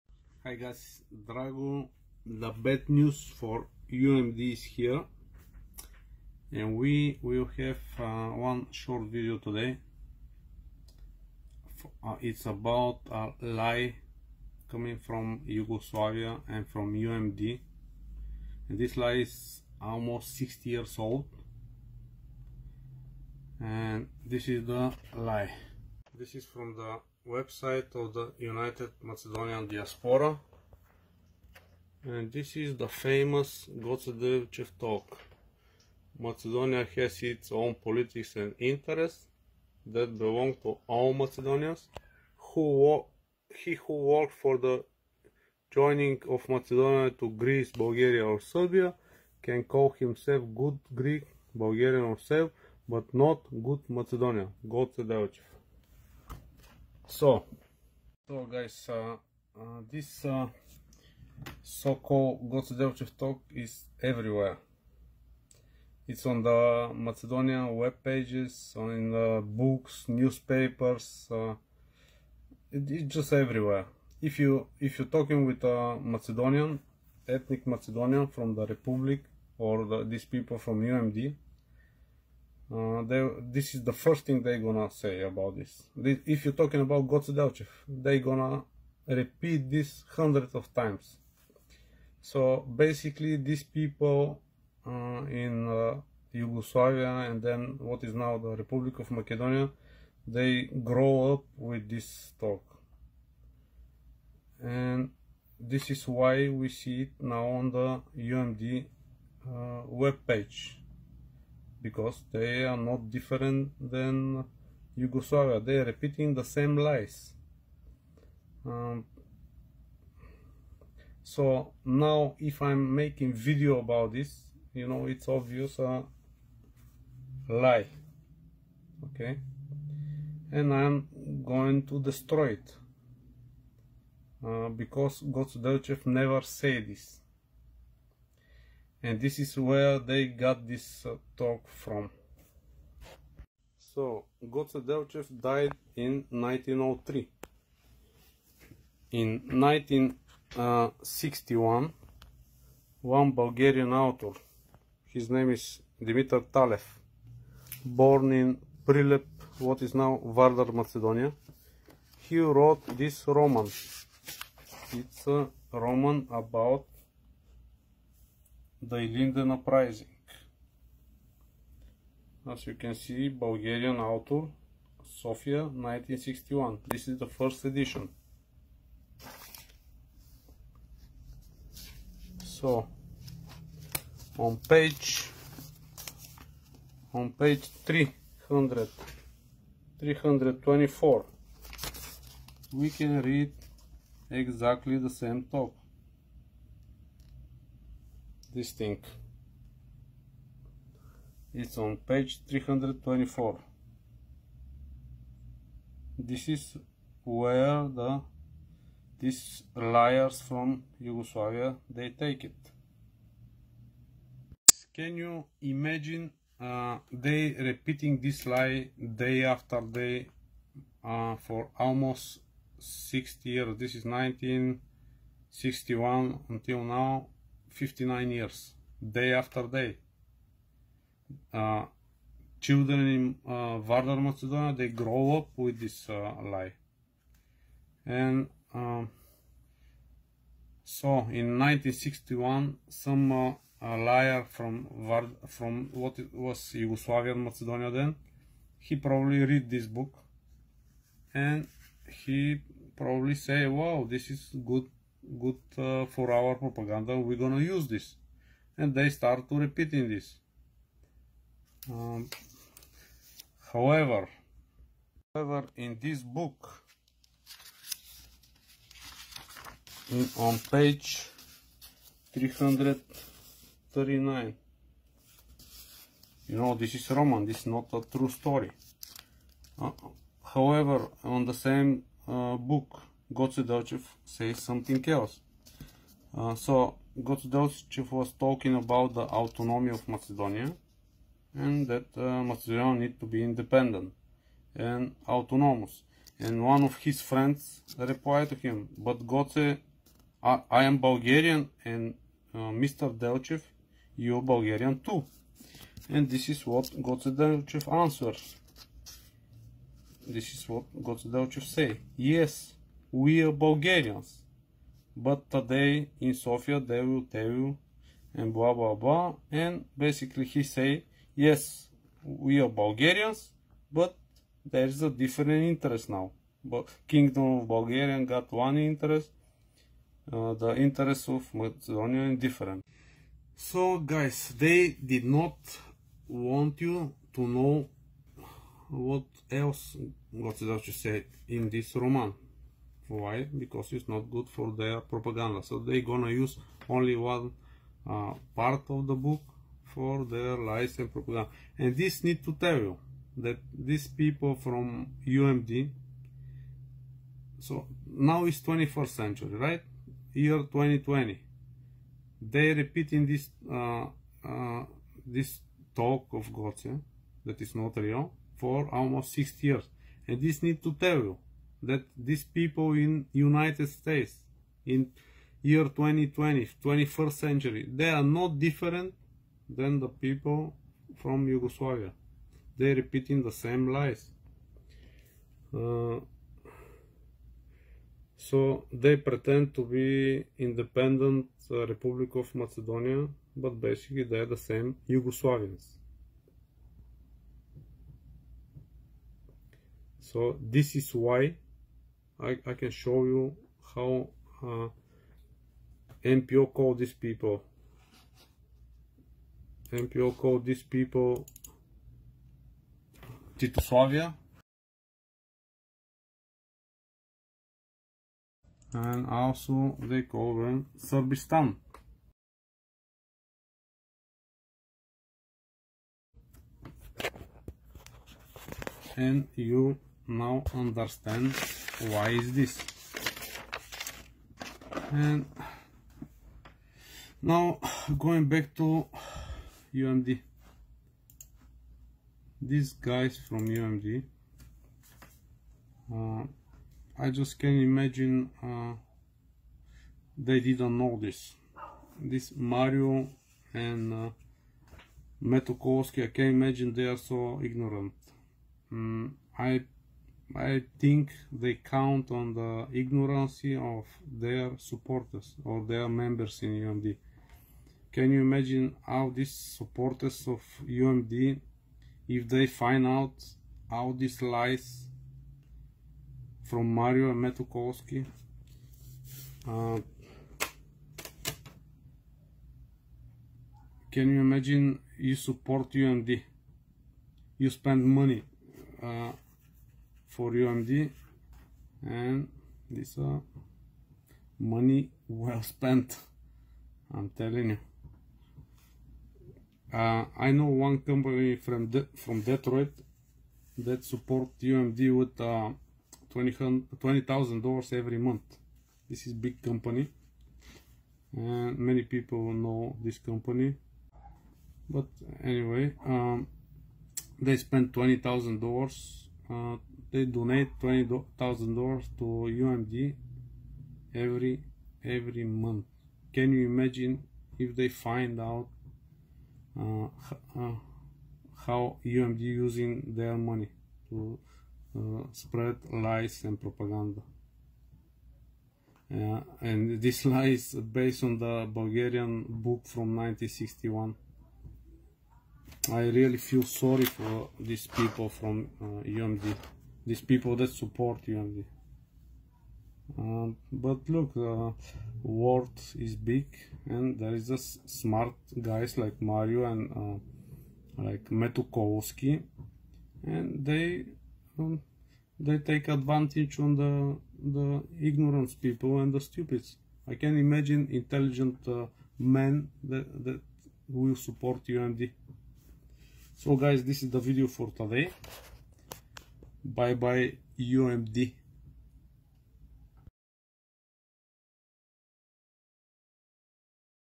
UMD Lie#9 Goce Delchev Thought
UMD ( United Macedonian Diaspora )Lie #9 . The famous Goce Delchev thought is actualy taken from the Bulgarian Roman(Novel) from 1961 "Ilinden" by the author Dimitar Talev.